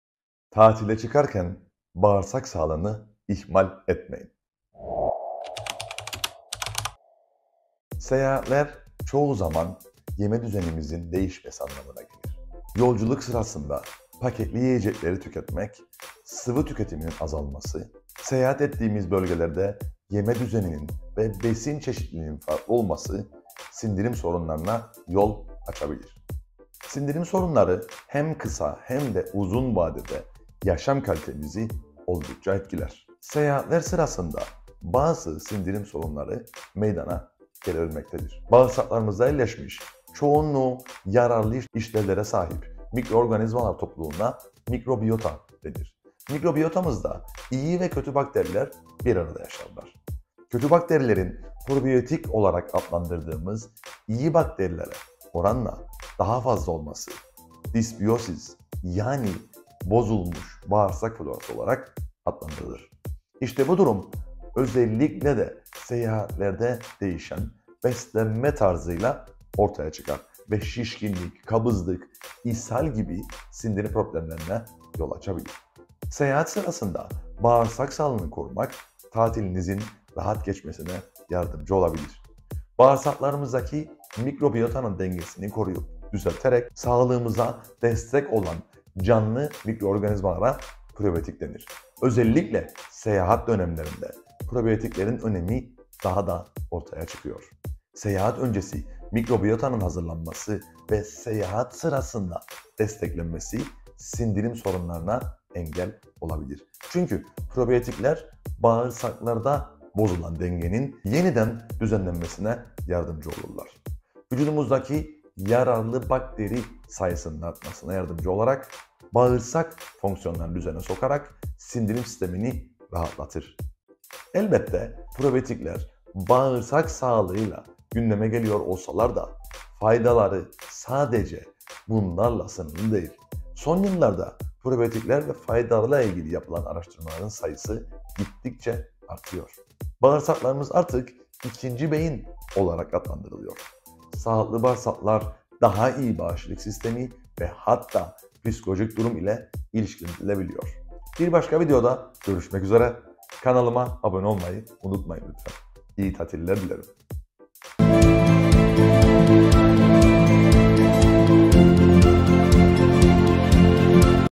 Tatilde ishal, şişkinlik, kabızlık !!
Tatil öncesi ve sonrası bağırsak sağlığı çok önemli. Oluşabilecek ishal ve kabızlık durumunu engellemek için beslenmeniz çok büyük önem kazanıyor.

Seyahat sırasında bağırsak sağlığını korumak tatilinizin rahat geçmesine yardımcı olabilir.

Tüm detayları ile anlatmaya çalıştım.

İyi seyirler…